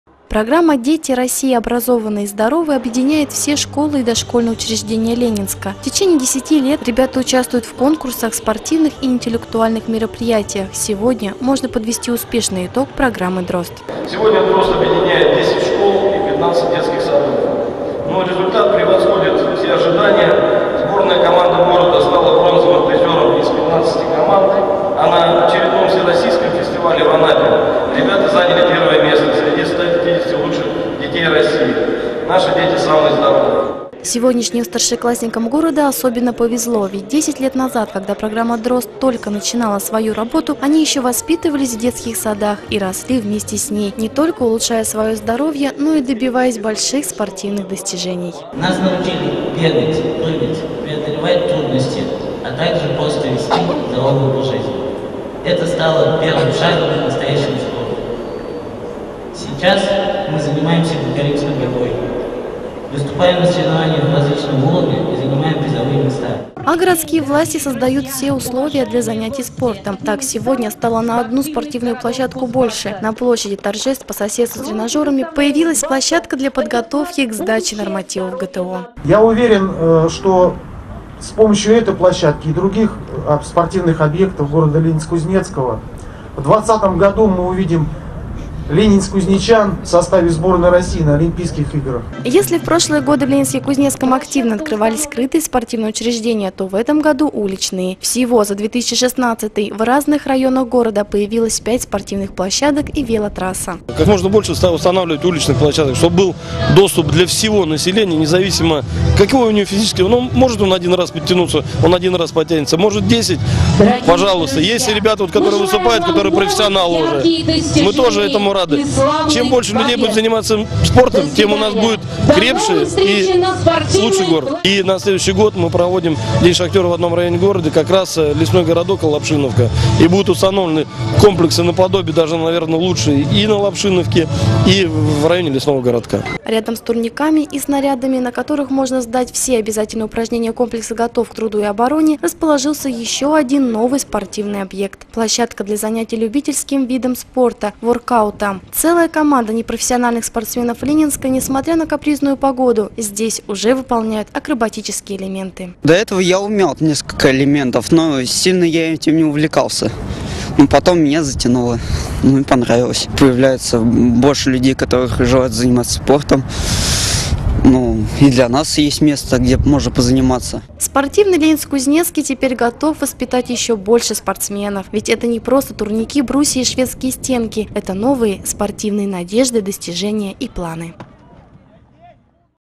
Программе «Дети России образованы и здоровы» 10 лет
В Физкультурно-оздоровительном Комплексе Ленинска-Кузнецкого прошел 10-ый юбилейный  городской физкультурно-образовательный фестиваль «Дети России образованы и здоровы». С юбилейной датой дошкольников и школьников города поздравили – заместитель начальника управления физической культуры и спорта Кемеровской области Александр  Боксгорн и мер Ленинска-Кузнецкого Вячеслав Телегин. Спортивный  праздник продолжился  на площади  Мазикина,  где   власти города  торжественно открыли очередную спортивную  площадку. Теперь и на площади торжеств есть специально предназначенная зона для подготовки выполнения норм ГТО. А также появилось новое оборудование, для любителей популярного  в  последнее  время  такого  вида  спорта, как  воркаут.